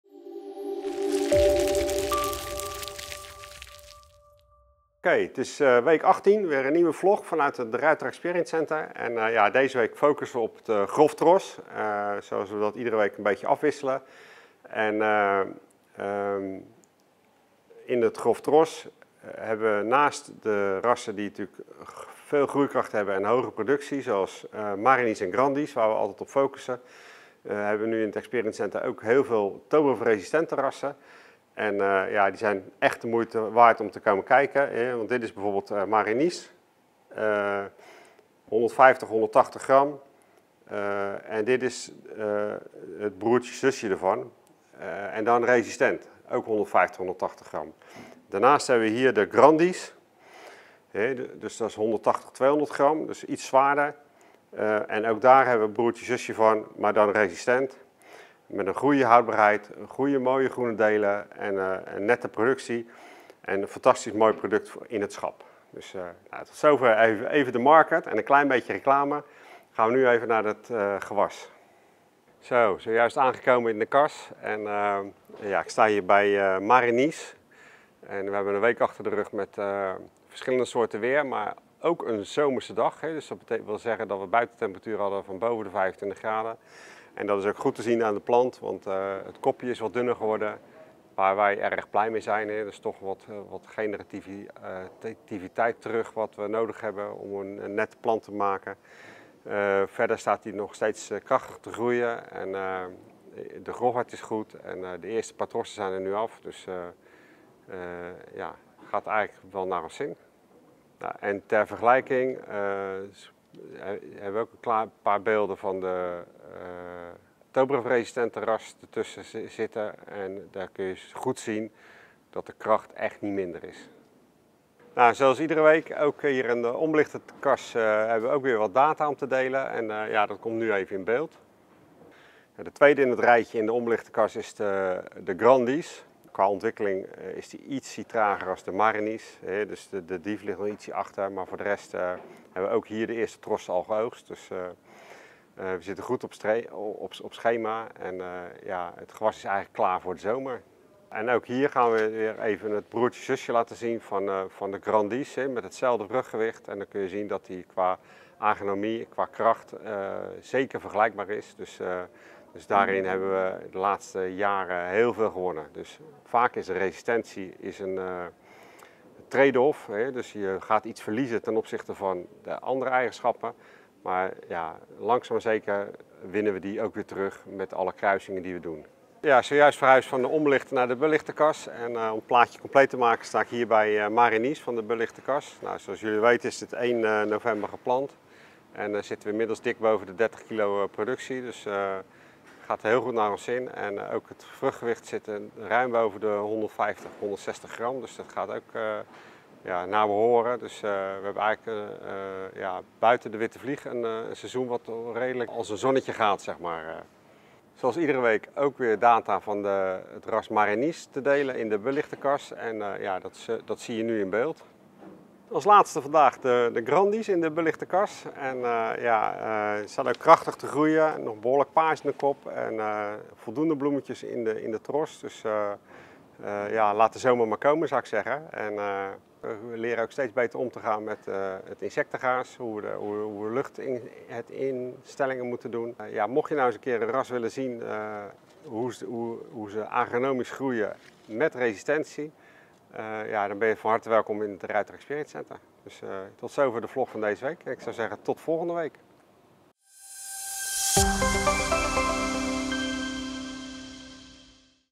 Virtual Tour De Ruiter Experience Center 3 mei 2024_NL (+ English subtitles)
Wij nodigen u met veel plezier uit voor een virtuele rondleiding door de De Ruiter-kassen in week 18 van 2024. In de video van deze week laat John van der Knaap, de manager van het De Ruiter Experience Center, u de ontwikkeling zien van tomaten in het grove segment, waaronder Marinice en Grandice. Vandaag begint John in de markt waar hij trots de ToBRFV-resistente broertjes en zusjes van Marinice en Grandice presenteert. Met de eerste mooie zomerdag achter de rug is de positieve invloed hiervan duidelijk zichtbaar op de planten. Laten we samen een kijkje nemen in de kas om dit van dichtbij te bekijken.
Blijf kijken voor meer updates en inzichten in ons teeltproces!